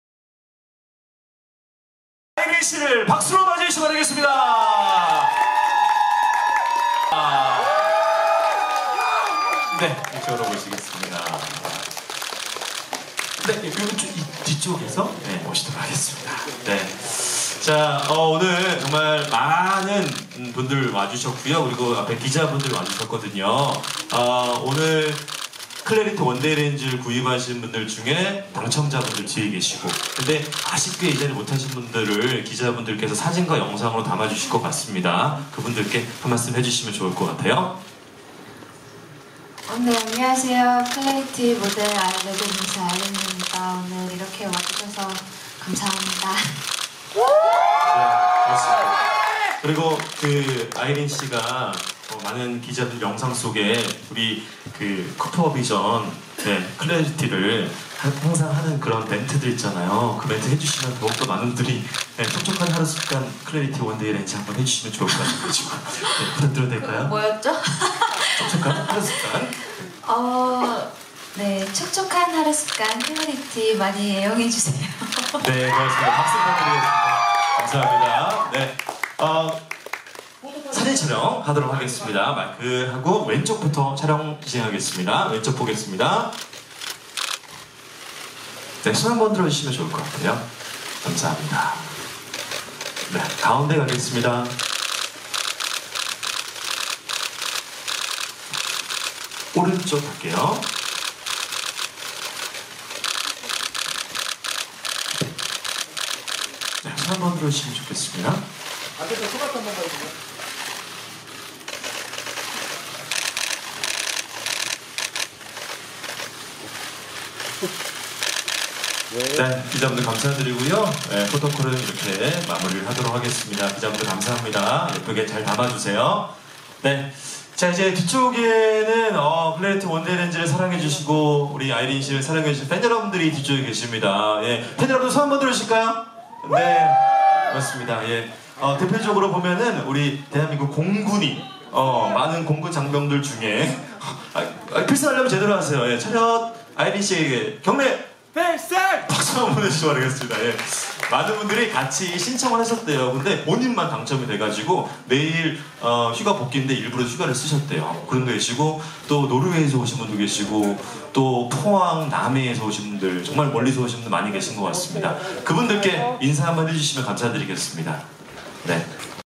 190529 레드벨벳 아이린(IRENE) 비교불가 절대 미모, 쿠퍼비전 코리아 팬사인회
29일 오후 서울시 강남구 청담동 일지아트홀에서 콘택트렌즈 브랜드 쿠퍼비전 코리아의 홍보대사인 아이린의 팬사인회 이벤트가 진행돼 레드벨벳 아이린이 참석해 포토타임을 갖고 있다.

이번 행사는 한달 간 이벤트 응모를 통해 선발된 100명의 팬들을 초대해 팬사인회와 더불어 팬들과의 소통을 위해 팬과 함께하는 미니 인터뷰도 진행됐다.